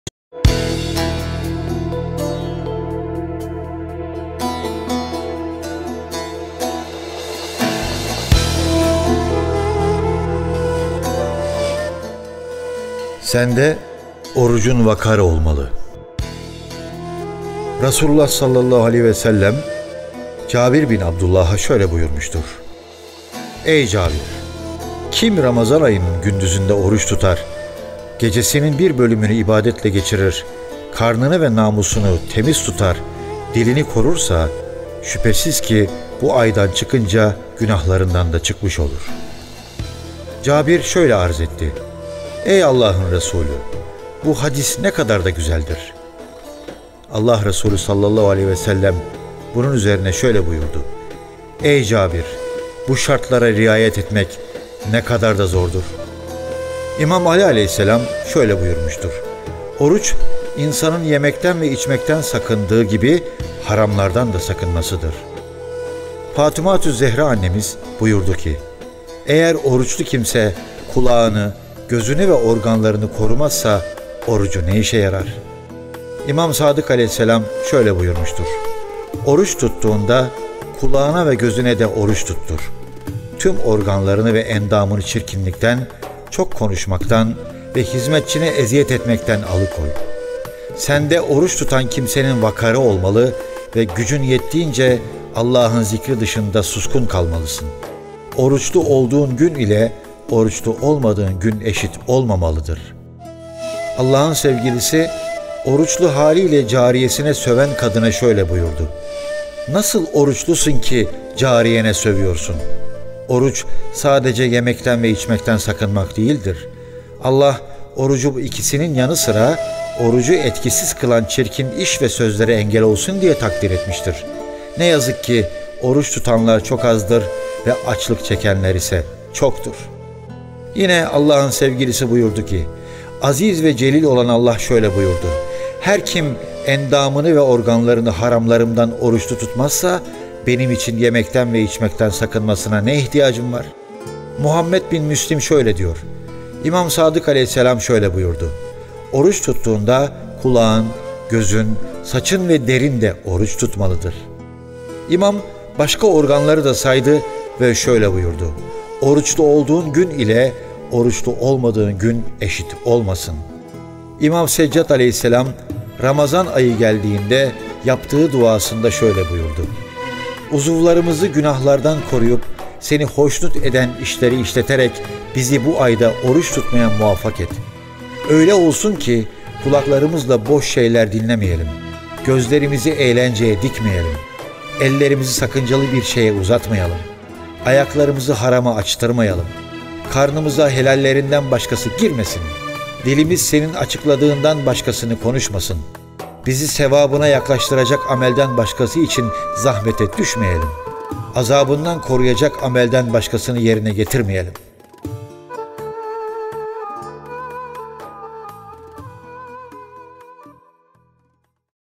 Ehlibeyt Günlüğü: ‘Sende orucun vakarı olmalı’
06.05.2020 tarihli Yeni Mesaj Gazetesinde yayınlanan Ehlibeyt Günlüğü

İmam Sâdık (a.s) şöyle buyurmuştur: “Oruç tuttuğunda kulağına ve gözüne de (haramlardan) oruç tuttur. Tüm organlarını ve endamını çirkinlikten, çok konuşmaktan ve hizmetçine eziyet etmekten alıkoy. Sende oruç tutan kimsenin vakarı olmalı ve gücün yettiğince Allah’ın zikri dışında suskun kalmalısın. Oruçlu olduğun gün ile oruçlu olmadığın gün eşit olmamalıdır”